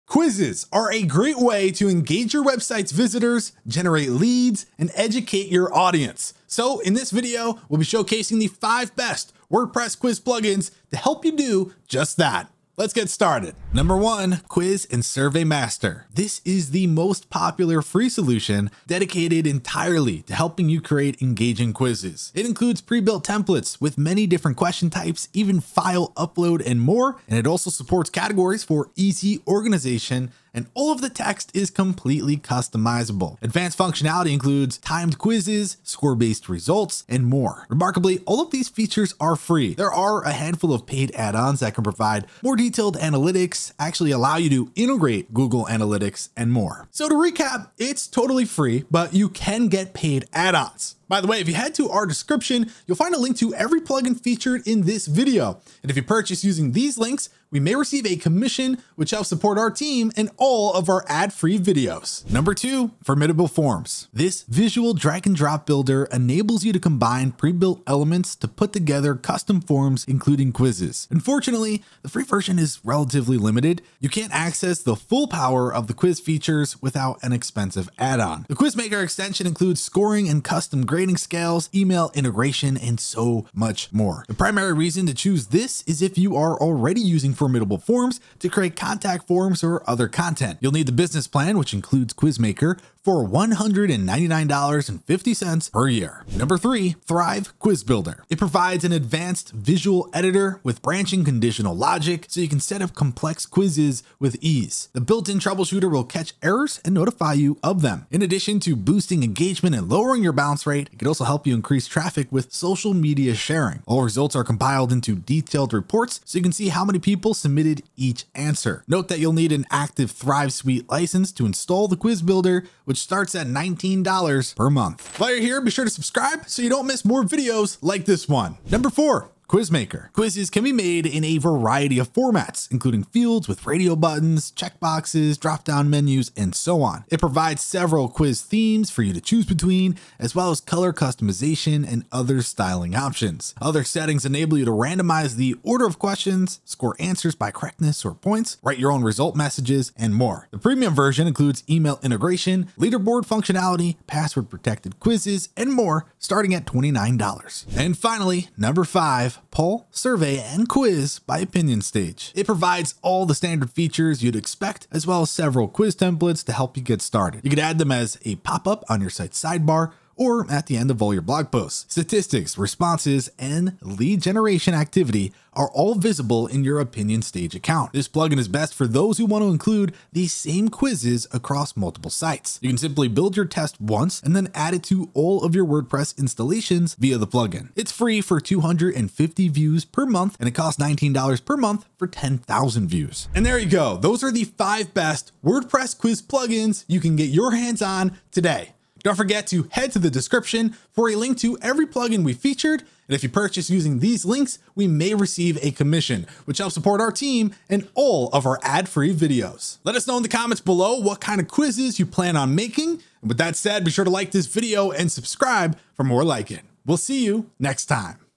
5 Best WordPress Quiz Plugins
Quizzes are a great way to engage your website visitors, generate leads, and educate your audience. With the help of WordPress quiz plugins, you can create engaging quizzes that can help you generate leads, increase engagement, and even educate your audience. In this video, we'll explore the top five WordPress quiz plugins that are leading the way in 2024. Whether you're a blogger, marketer, or an e-learning website owner, these plugins are sure to help you create exciting and informative quizzes. So, let's dive in and discover the best WordPress quiz plugins of 2024!

Timestamps ⏰
0:00 Intro
0:13 Quiz And Survey Master
1:02 Formidable Forms
1:36 Thrive Quiz Builder
2:08 Quiz Maker
2:37 Poll, Survey & Quiz by OpinionStage
3:13 Outro